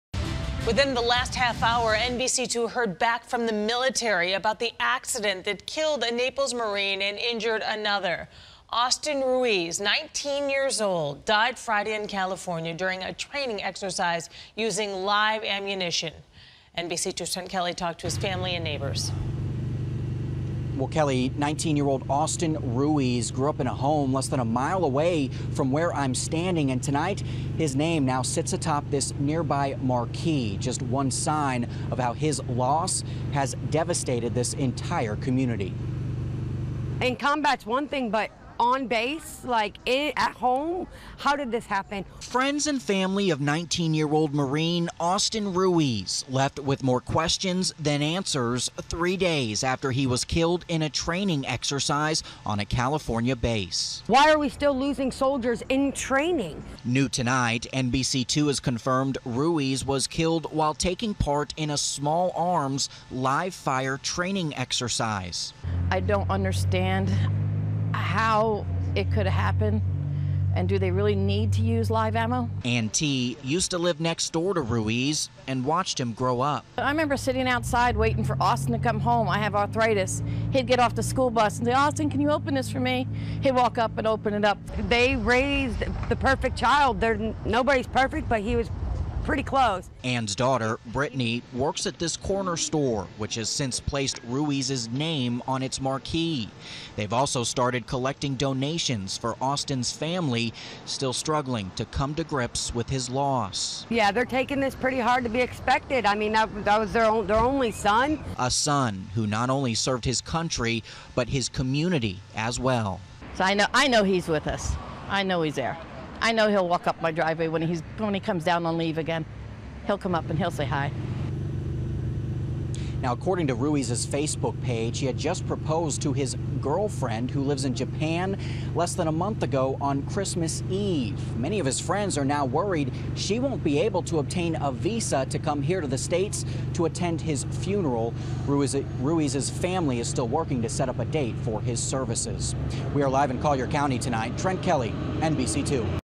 Naples Marine died during live-fire training
A 19-year-old Marine from Collier County was killed during live-fire training, NBC2 has confirmed.